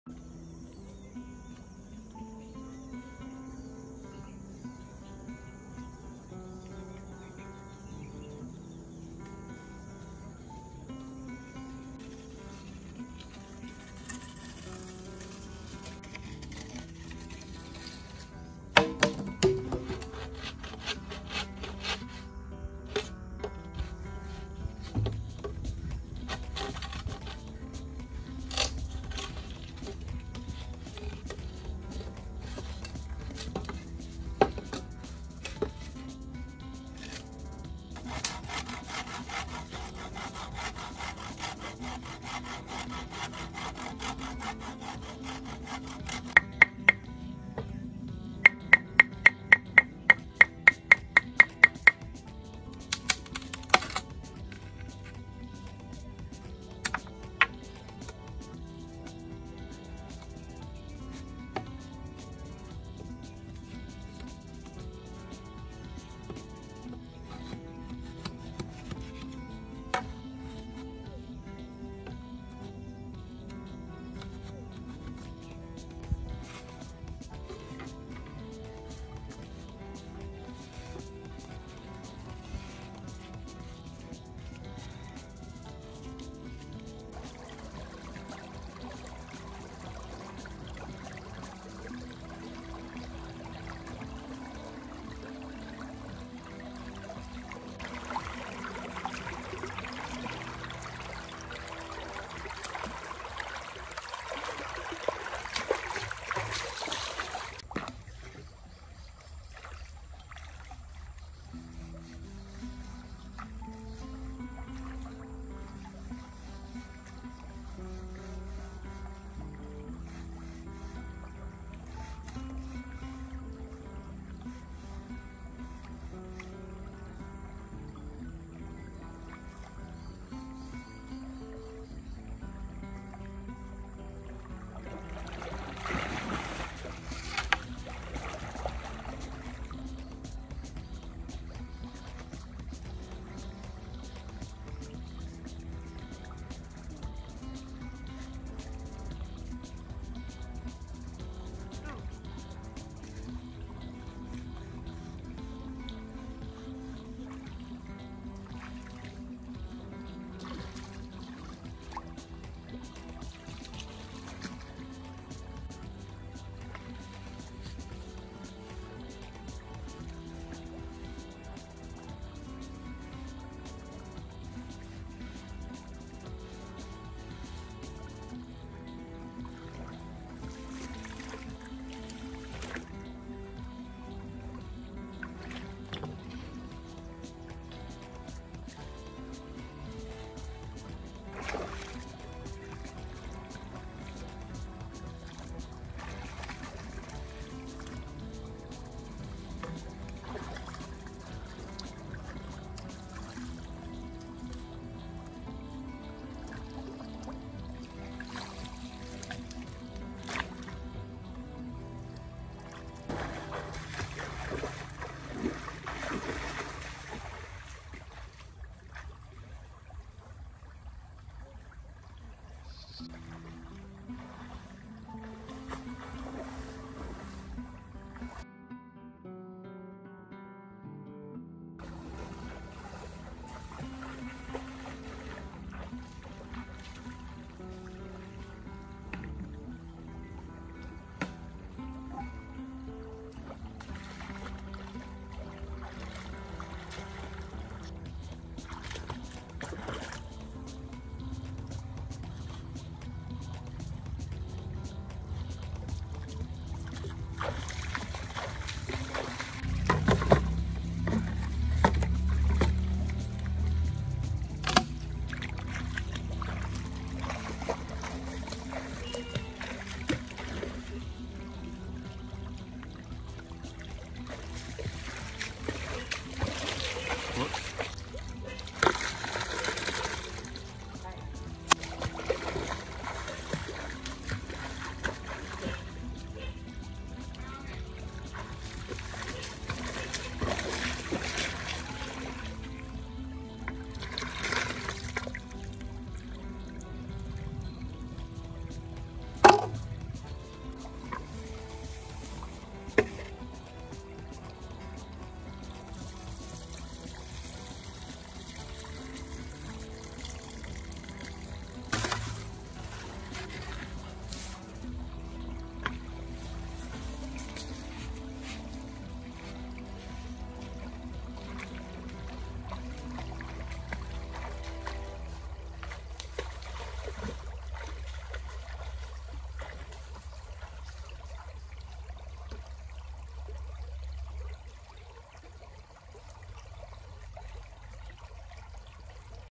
Beautiful girl video catch eel with bamboo
I wonder how the fish and eel go inside the bamboo? Actually the girl made a hold from bottom of the bamboo, and put some foods at the bottom. Fish smelling the food, and find the way to go inside Bamboo.

I also never seen how people catch fish and eels from this kind of Bamboo, but she did it.

Primitive Life is now publishing a personal blog for fun. Supporting us 1000 subscribers within a month to growth more videos